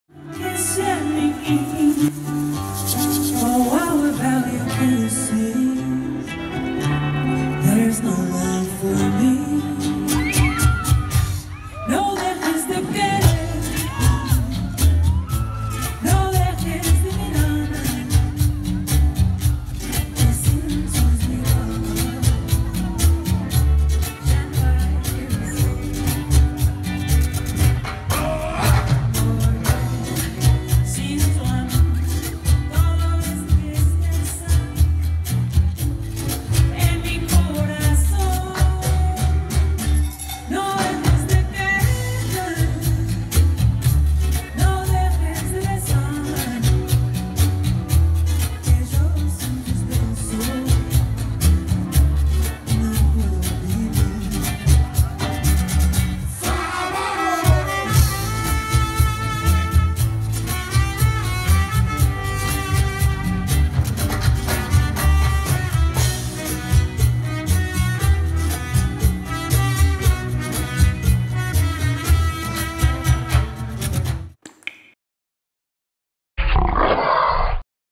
PATZCUARO CANTOYA FEST 2016 promo FESTIVAL 2017
Michoacan México en el 2017 regresa El Cantoya Fest

Un festival Internacional de Globos de papel , cultura y mas !